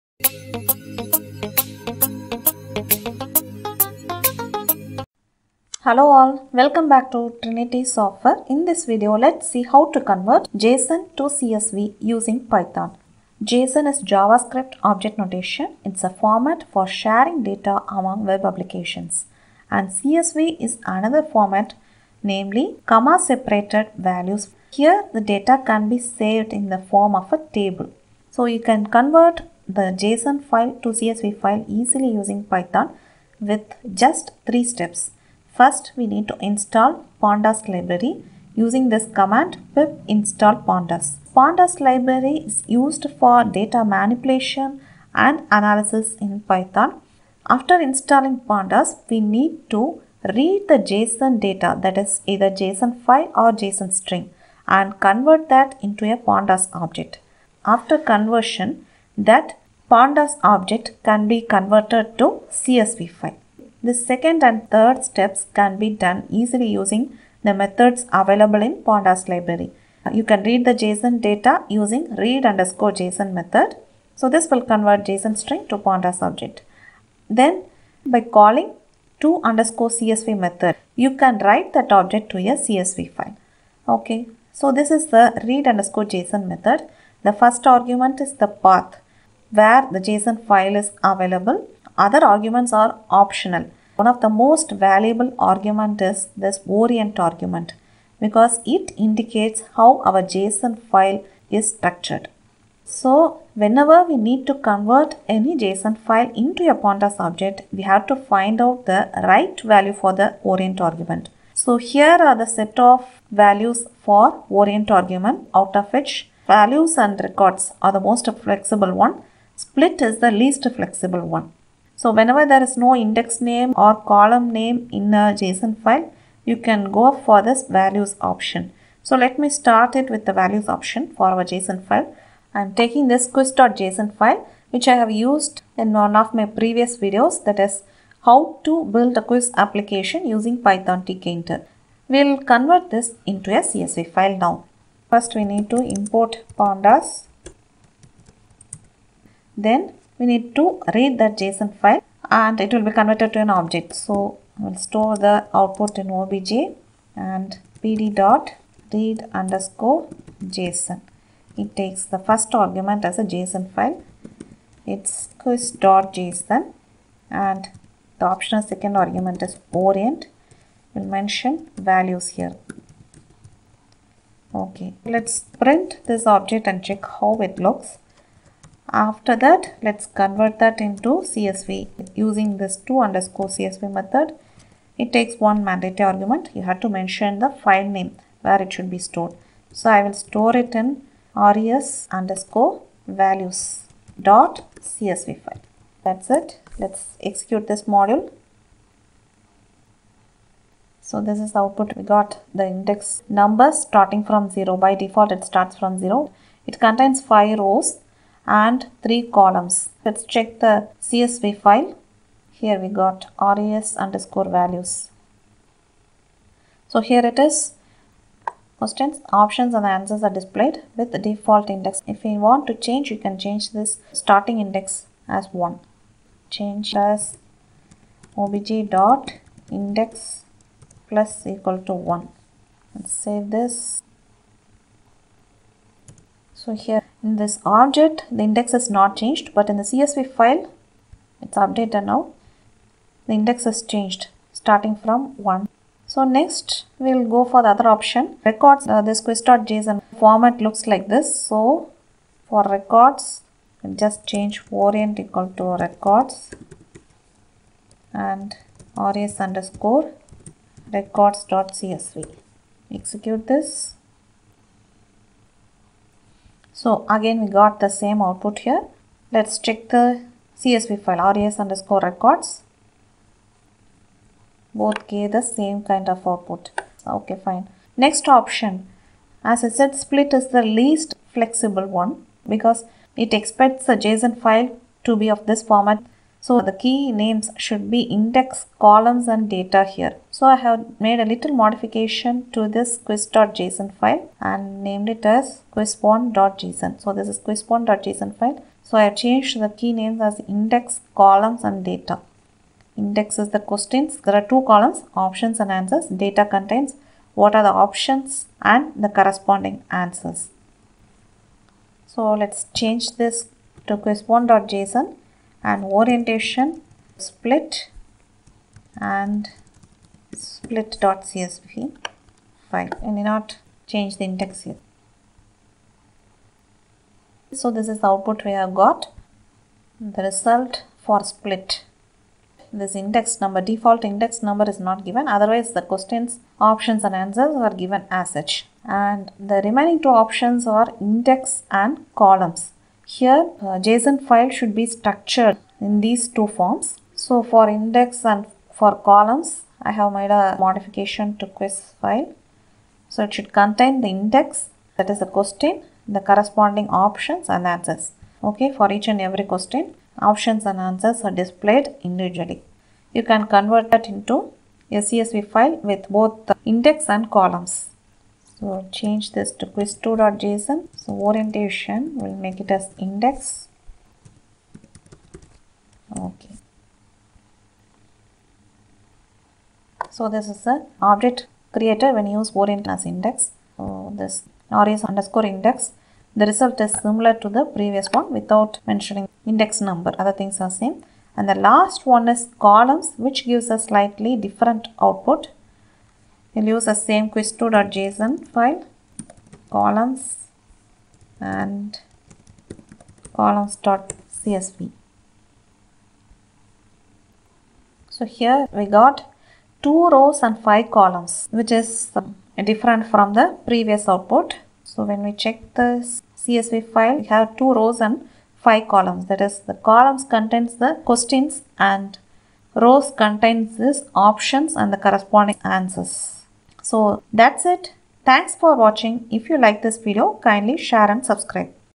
how to convert JSON to CSV using Python
This video explains how to convert JSON file to CSV file using Pandas library in Python.
3 different formats of JSON files are taken and converted to CSV file by changing the value of orient argument in read_json() method of pandas.